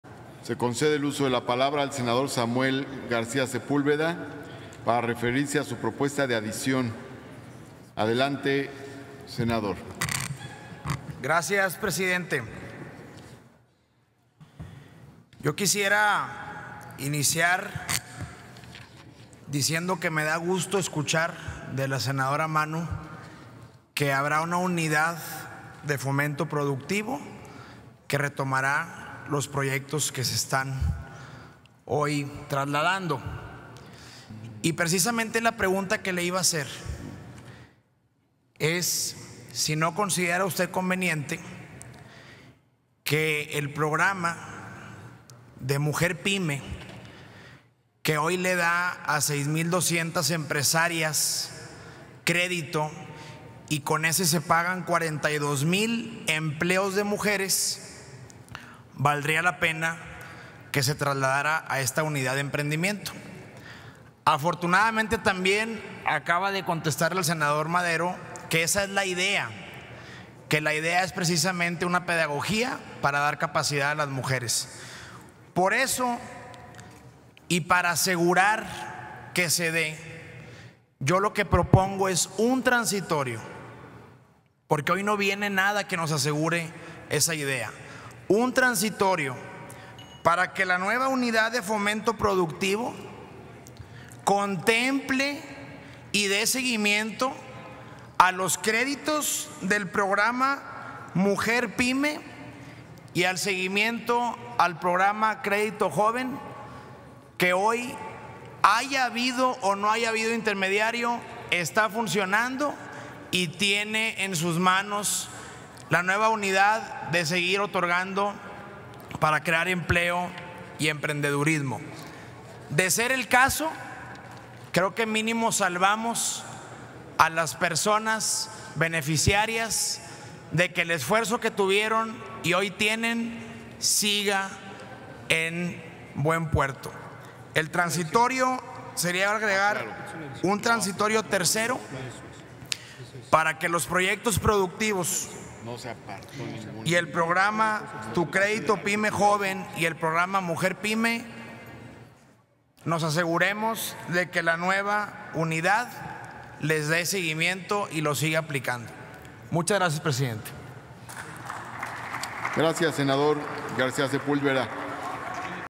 Sen. Samuel García pide que continúe programa de Mujer PyME
El senador Samuel García, del Grupo Parlamentario de Movimiento Ciudadano, presenta reservas al voto particular del dictamen que desecha el proyecto de decreto que reforma y deroga diversas disposiciones de la ley para el desarrollo de la competitividad de la micro, pequeña y mediana empresa.

Señala que ese programa del Instituto Nacional del Emprendedor da créditos actualmente a 6 mil 200 empresarias, con lo que se pagan 42 mil empleos de mujeres. Pide trasladarlo a la nueva Unidad de Fomento Productivo, junto con el Crédito Joven.